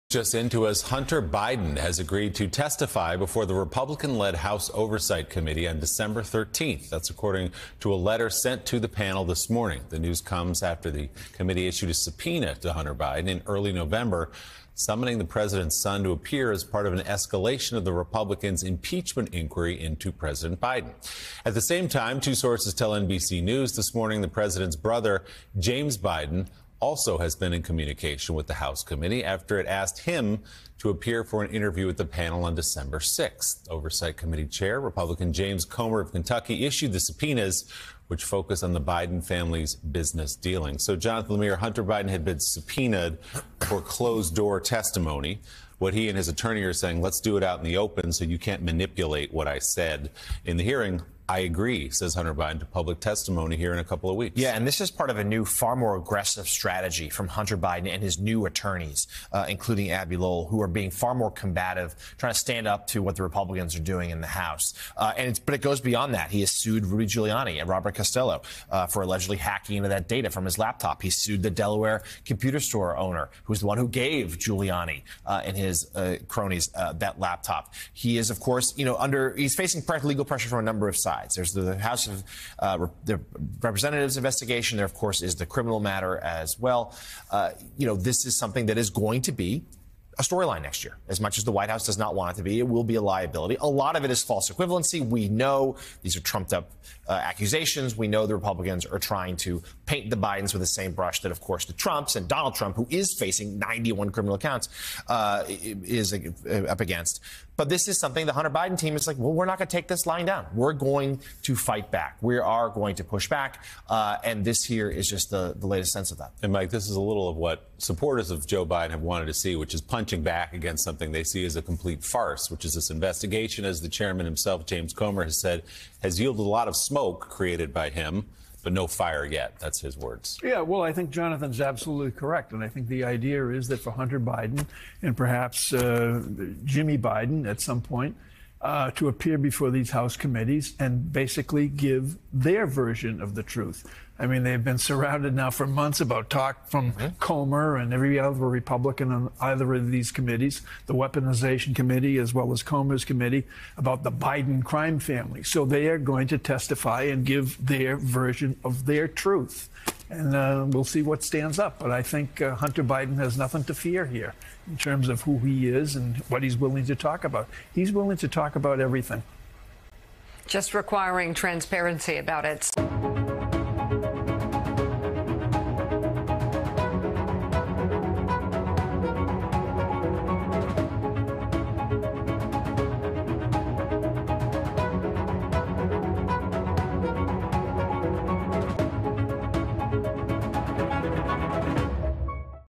Hunter Biden agrees to testify before GOP-led House Oversight Committee
Hunter Biden is open to testifying publicly before the Republican-led House Oversight Committee on Dec. 13 in a letter his lawyer sent to the panel Tuesday.